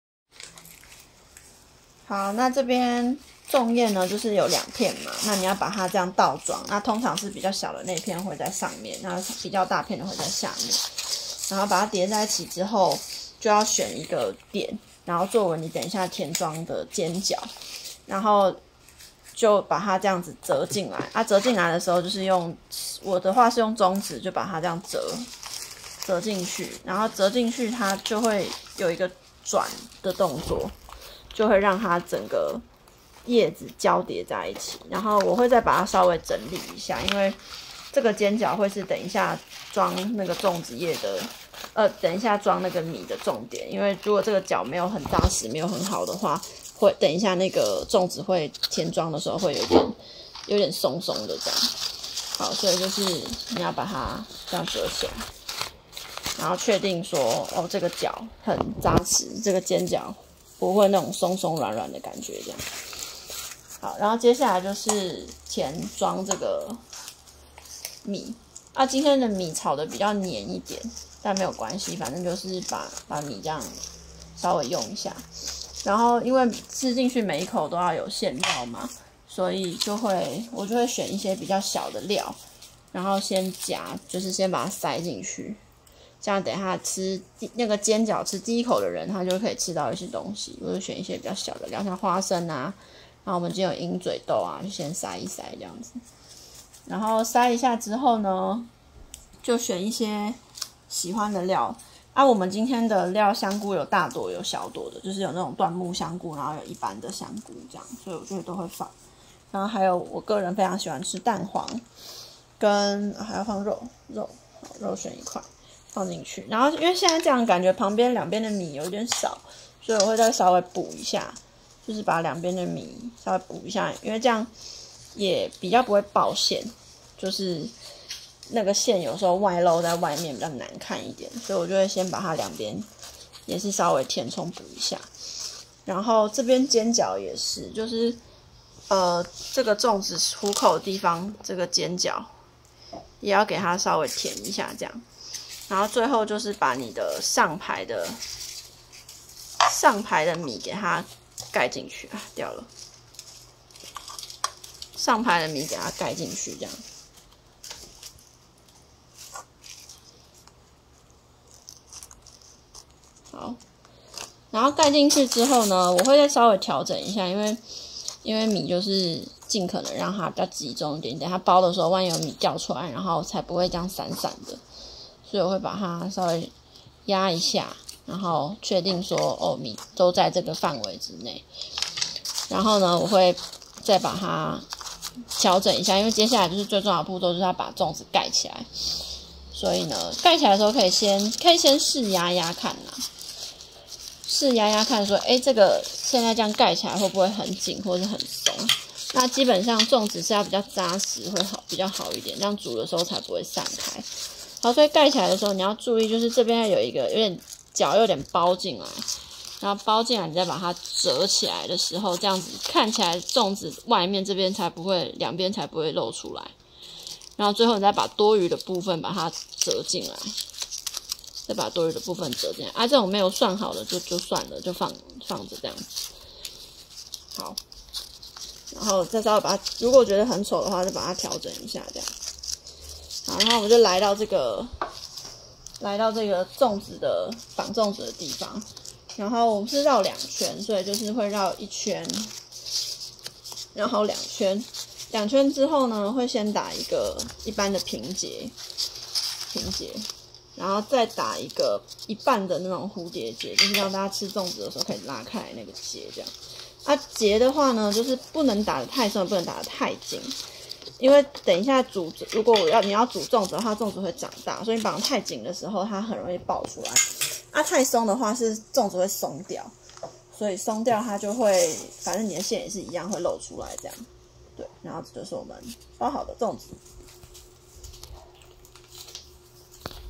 包粽子 簡單學 端午節 輕鬆來包粽 不露餡 關鍵手法詳細解說 五分鐘學起來
年輕人也學得會包粽子 關鍵手法五分鐘學會